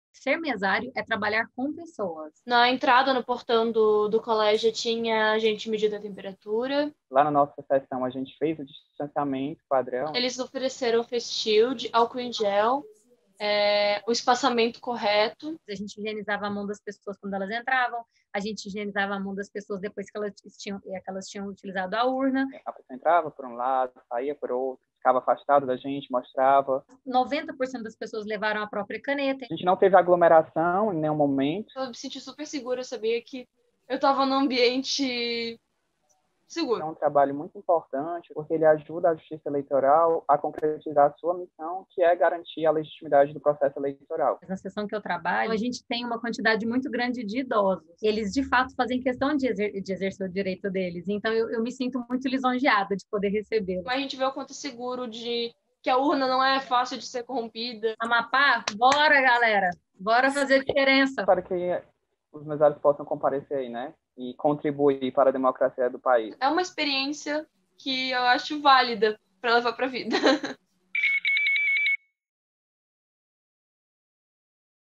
Orgulho de ser mesário
Mesários falam sobre a importância do trabalho nas seções eleitorais e destacam os protocolos seguidos nas Eleições Municipais 2020 para evitar a disseminação do novo coronavírus.